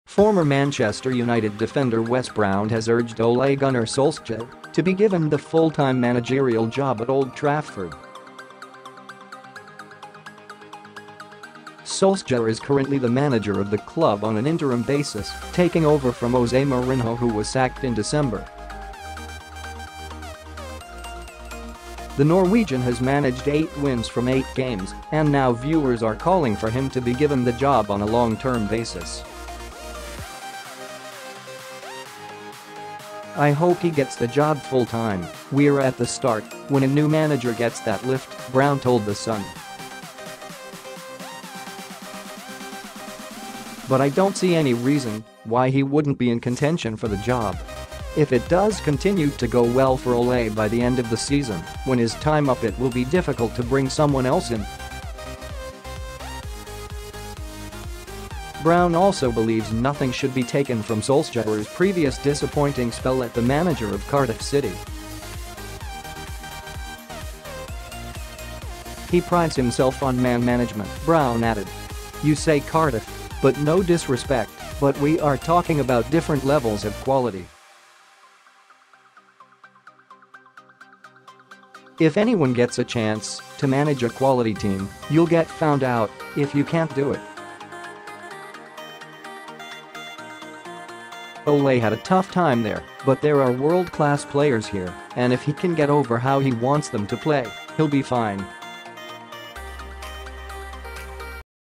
Manchester United need to hire Ole Gunnar Solskjaer permanently, says Wes Brown
Former Manchester United defender Wes Brown has urged Ole Gunnar Solskjaer to be given the full-time managerial job at Old Trafford.  Solskjaer is currently the manager of the club on an interim basis, taking over from Jose Mourinho who was sacked in December.  The Norwegian has managed eight wins from eight games and now viewers are calling for him to be given the job on a long-term basis.  ‘I hope he gets the job full time, we’re at the start when a new manager gets that lift,’ Brown told The Sun. ‘But I don’t see any reason why he wouldn’t be in contention for the job.  ‘If it does continue to go well for Ole by the end of the season when his time up it will be difficult to bring someone else in.’    Brown also believes nothing should be taken from Solskjaer’s previous disappointing spell at the manager of Cardiff City.  ‘He prides himself on man management,’ Brown added. ‘You say Cardiff, but no disrespect, but we are talking about different levels of quality.  ‘If anyone gets a chance to manage a quality team, you’ll get found out if you can’t do it.  ‘Ole had a tough time there, but there are world class players here and if he can get over how he wants them to play, he’ll be fine.’